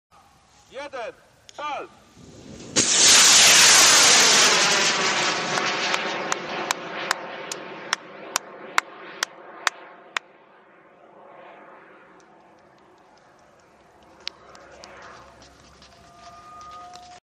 K1-X z hybrydowym silnikiem - film Kosmicznej Polski
Kolejny materiał ze startu krakowskiej hybrydy. Lot na 2,7 km na poligonie w Toruniu.